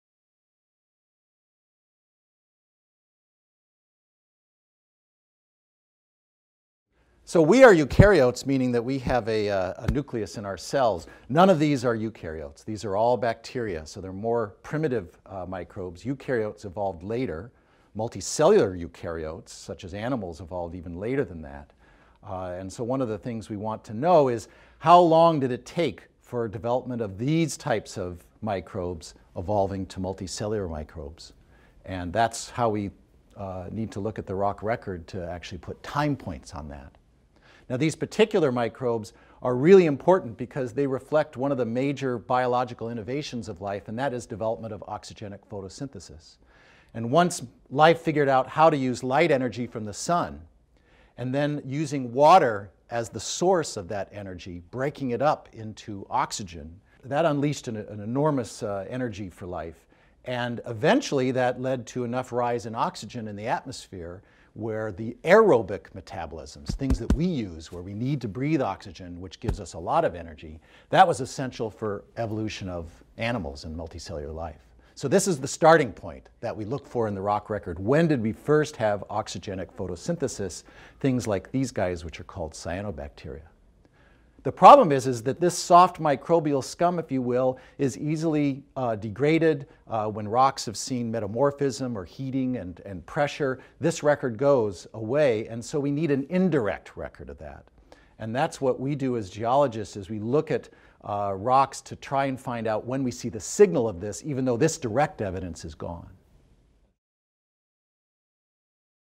Origins 2.3 Microbial Evolution
The very origins of life arose billions of years ago on Earth. Scientists at UW-Madison are studying how it began, from the first simple microbes that appeared early in Earth’s history, to the explosion of complex life just 600,000 years ago. Geology Professor Clark Johnson explains how scientists believe it may have all started.
"Origins" Co-created and Produced by Justin Bomberg